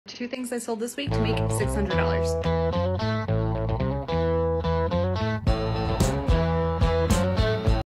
1 Week = $600: What I Flipped
These are two things I sold this week to make $600 through furniture flipping.
NOTE: This was not the profit, this is revenue.